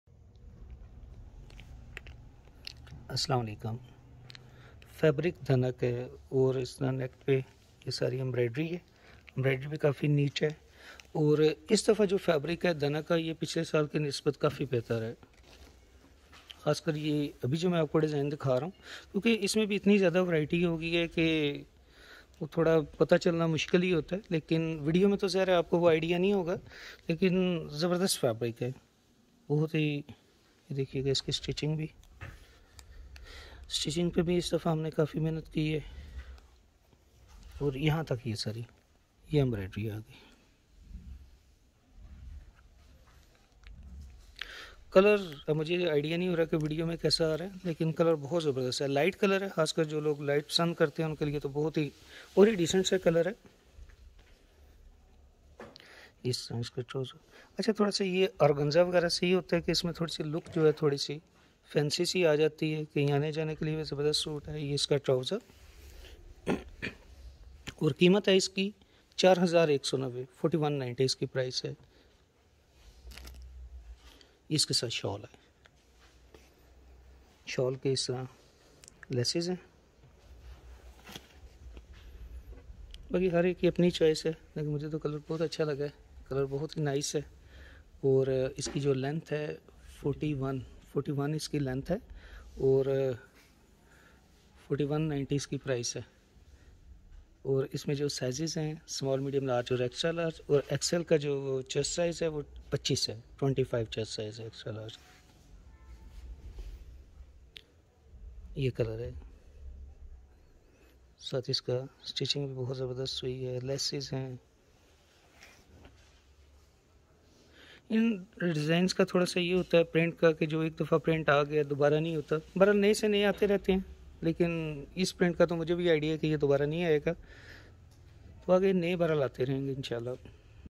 Winter Collection For Ladies🤩 \ New Fabric Dhanak Winter Dress🤩 \ Winter Stitched Dress Design
❄️ Winter just got a whole lot cozier with our enchanting Winter Collection for Ladies! Get ready to be swept off your feet by the elegance of Dhanak Winter Dress in our New Fabric. 🌟 Our Winter Stitched Dress Designs are a perfect blend of style, comfort, and warmth to keep you fashionably snug all season long.

👗 Dive into a winter wonderland of trendy and exclusive designs that will make you look and feel stunning, no matter how cold it gets. These dresses are perfect for casual outings, work, and those special winter events.

🛍️ Elevate your winter wardrobe, and make a bold fashion statement.

𝗡𝗼𝘁𝗲: Availability may vary. Check our website for the latest arrivals and stock information.

 If you're looking to revamp your winter wardrobe, you've come to the right place. Explore our stunning collection and stay stylish and cozy this winter.